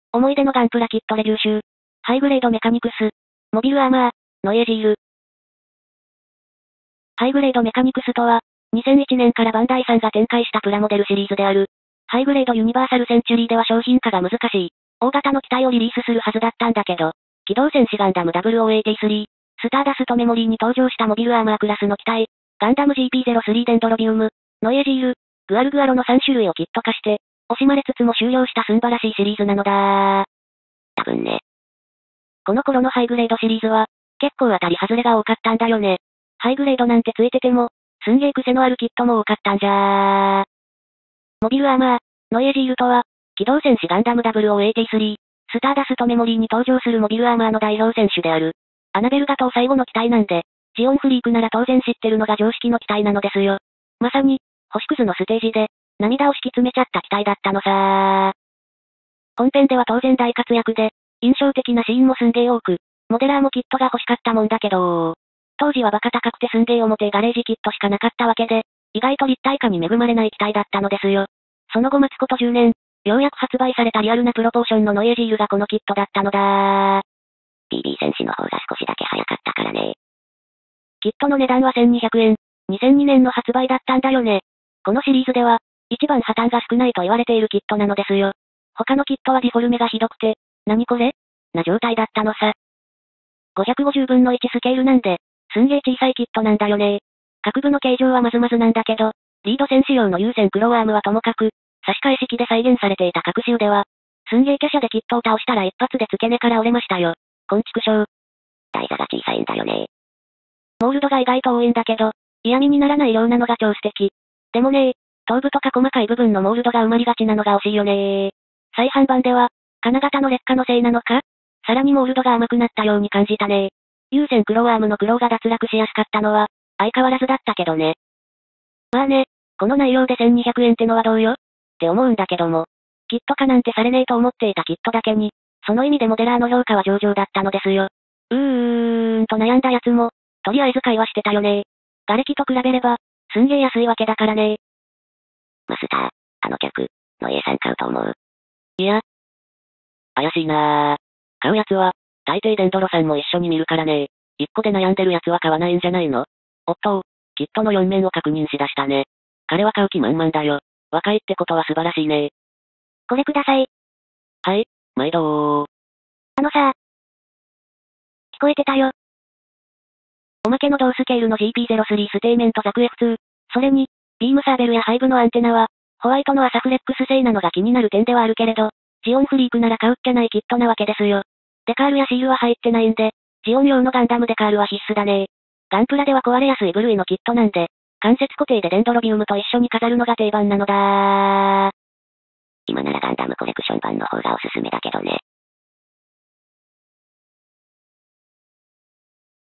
思い出のガンプラキットレビュー集 No.435 ☆ HGメカニクス 1/550 AMX-002 ノイエ・ジール Gundam Plastic Model Memories
[説明]
MAノイエジールとは機動戦士ガンダム0083スターダストメモリーに登場するMAの代表みたいな機体だよ。
アナベル・ガトーの最後の搭乗機なんで、ジオンフリークなら当然知ってるはずの機体なのですよ。
まさに、星屑のステージで涙を敷き詰めちゃった機体だったのだー!! .

◆最新のプラモデル↓